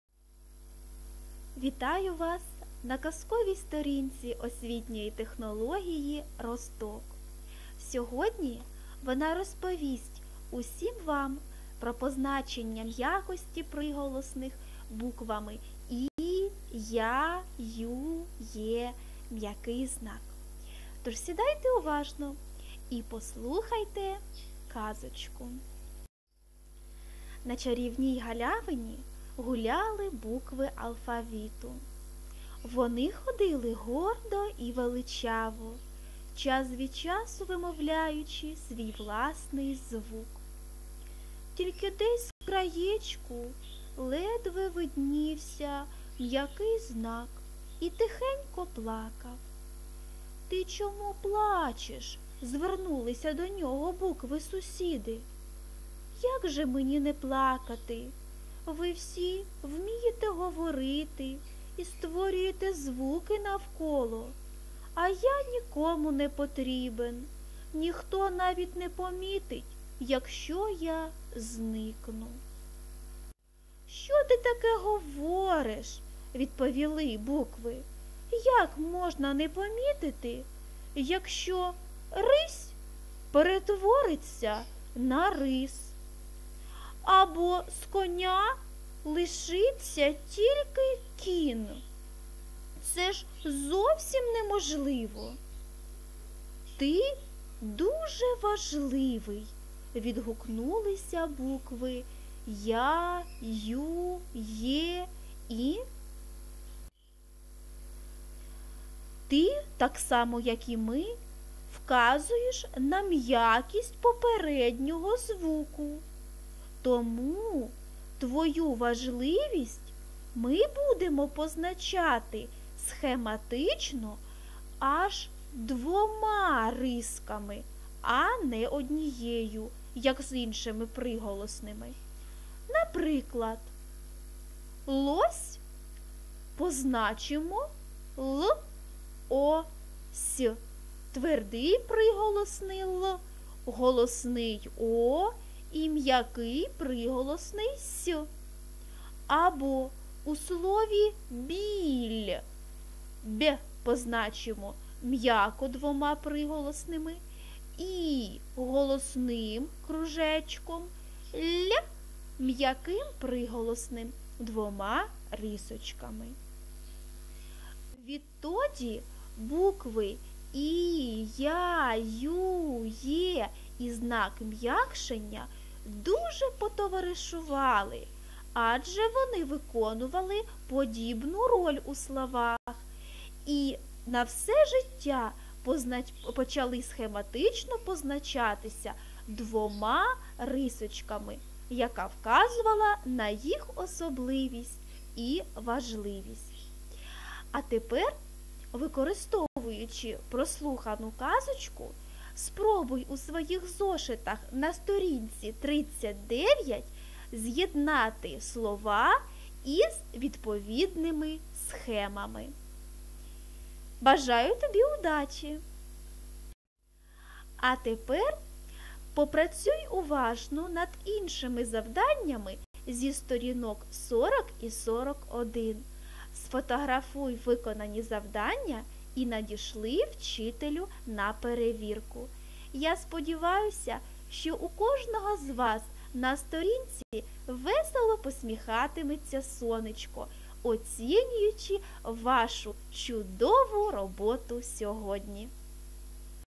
Позначення м'якості приголосних буквами і, я, ю ,є ь
Казка  про  позначення  м'якості  приголосних  звуків = ( за  осв.технологією "Росток")